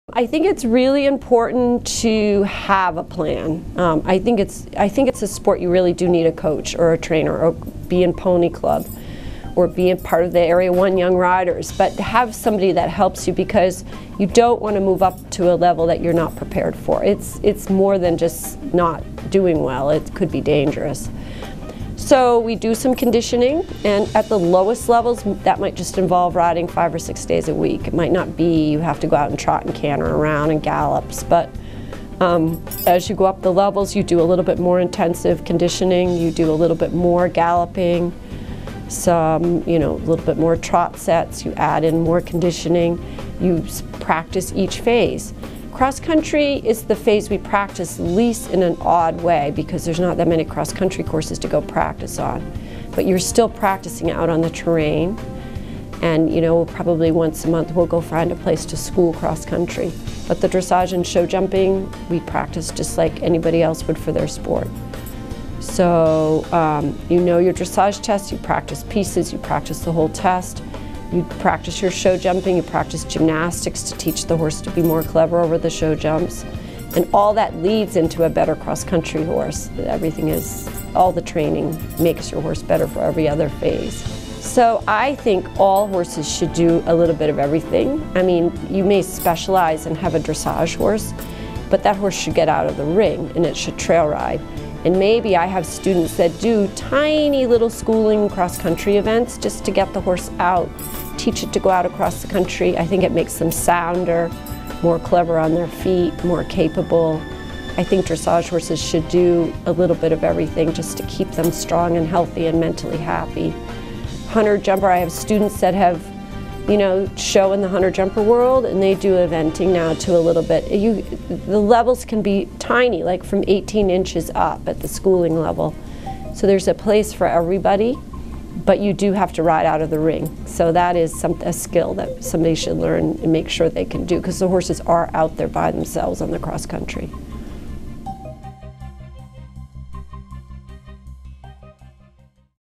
How to Prepare for Eventing
Jane Hamlin, Rider Trainer and FEI Eventing Judge, explains how to prepare the horse and rider for eventing, and why eventing is good for all horses.